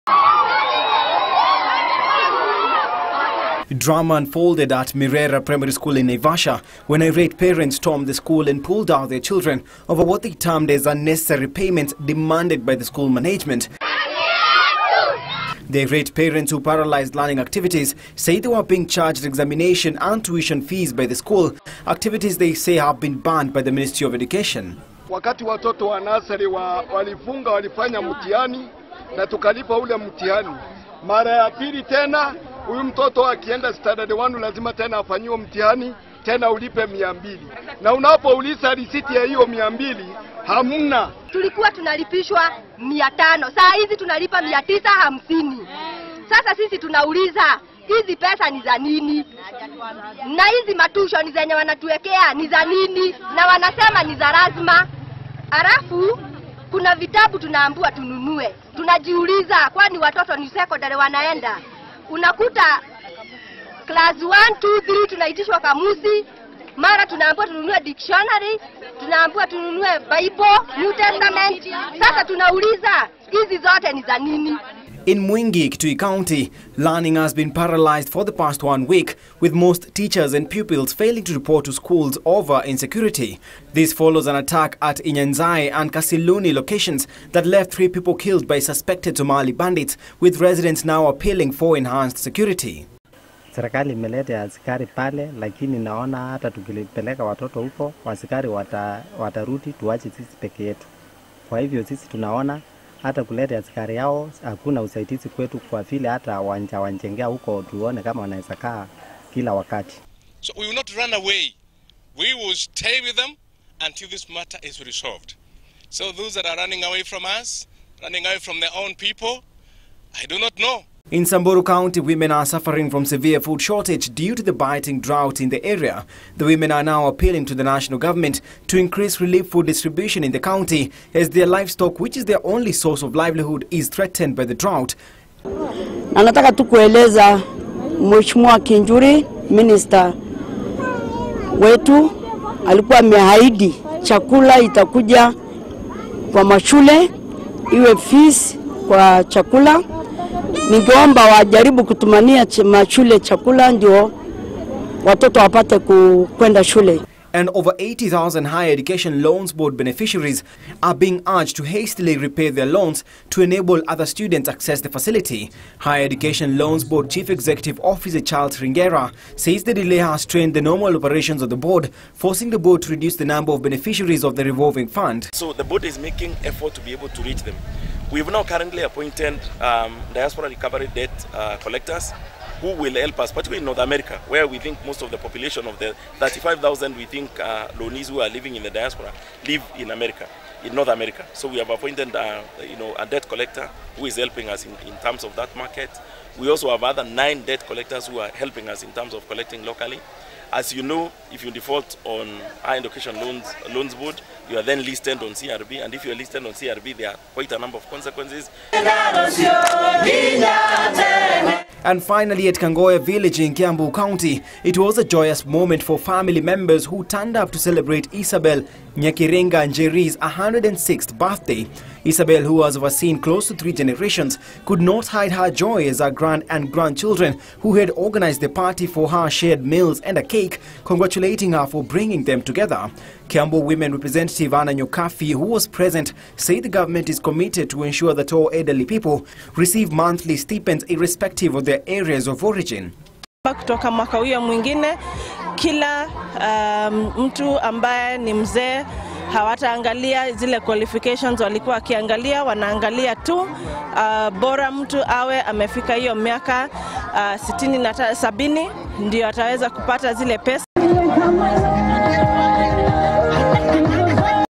Naivasha parents storm school in protest over extra fees
Learning was paralyzed at  Mirera Primary School in Naivasha after parents stormed the school demanding for an explanation from the head teacher  on why the school management is forcing them to pay monies for activities that were banned by the Ministry of Education. Elsewhere in Mwingi, teachers and pupils have failed to report to schools fearing for their lives following an attack at Inyanzae and Kasiluni locations that left three people killed by alleged Kenyan-Somali bandits.